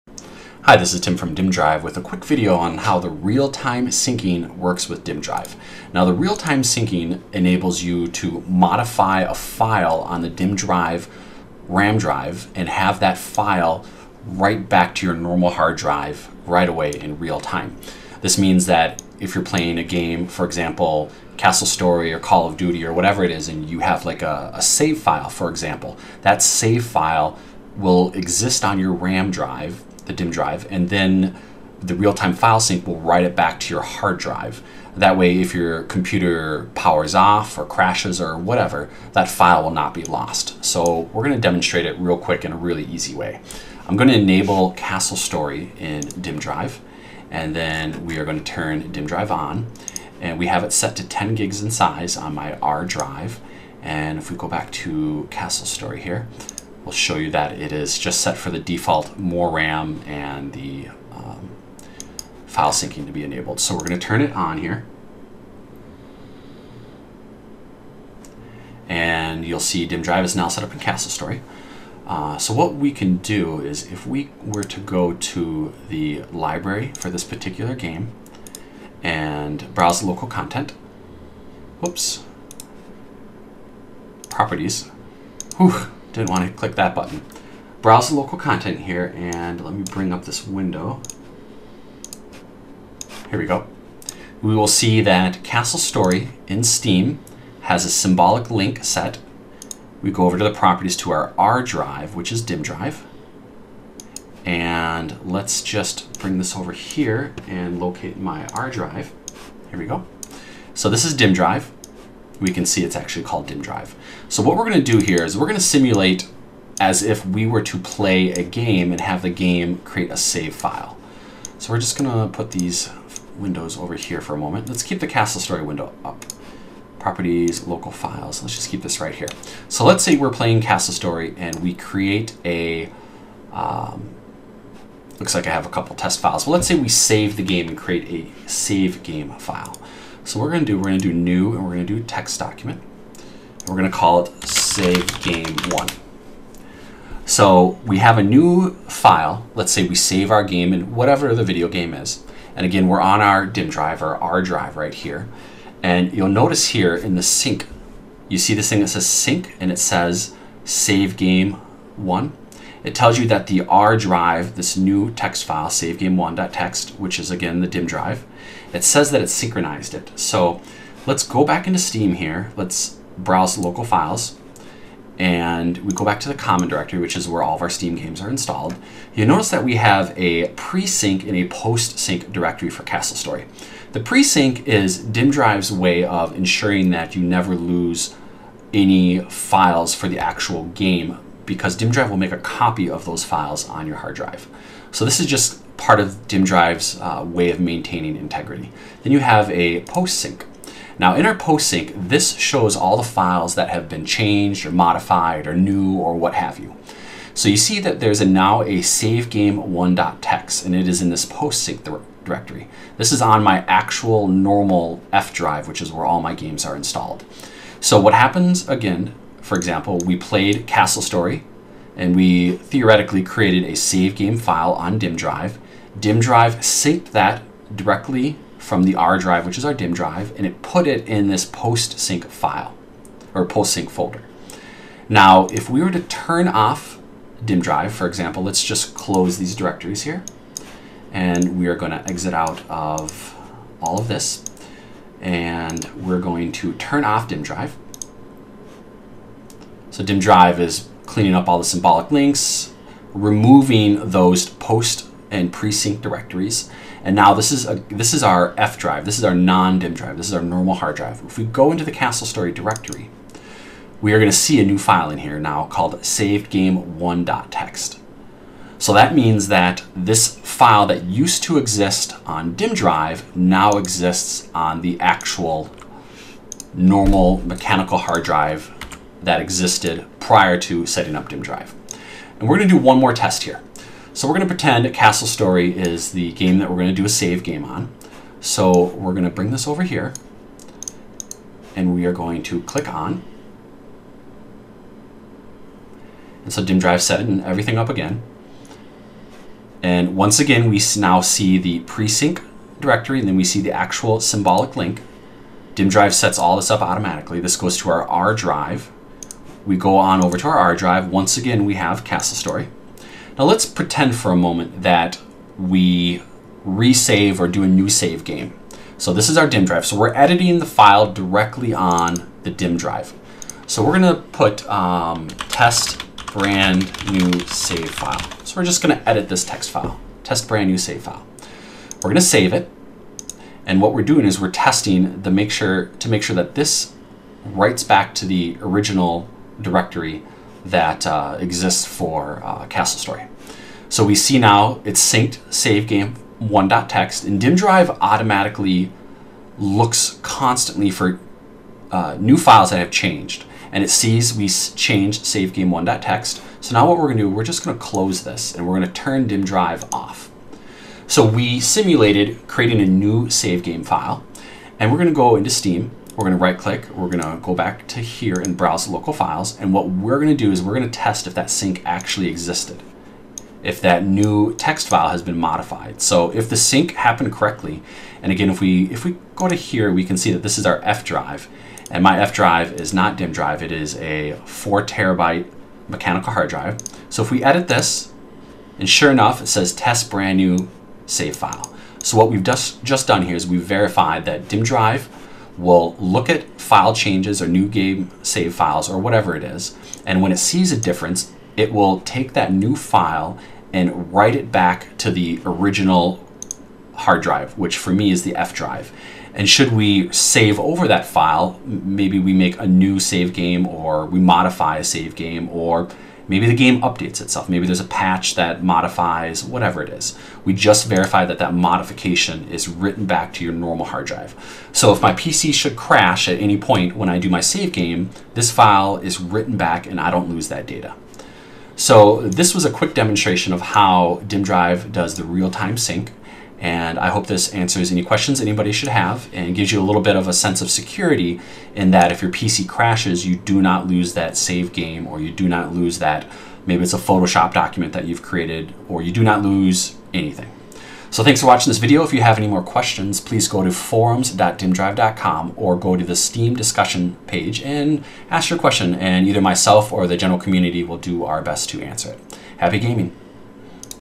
Dimmdrive's Real Time Sync Explained
This video shows the ramdrive real-time file synchronization feature in action.  This feature ensures that data is never lost should your PC power down unexpectedly.  Your save-game files are safe with Dimmdrive :)

ramdisk software answers that age-old issue of ramdrive's losing data when your pc shuts down/reboots/crashes by having real-time file syncing between your RAM drive and your hard drive.

Dimmdrive is a ramdrive built exclusively for gamers, with direct integration into your Steam games, as well as support for other games and programs.  Our program is brand new - come check us out @ and say hello!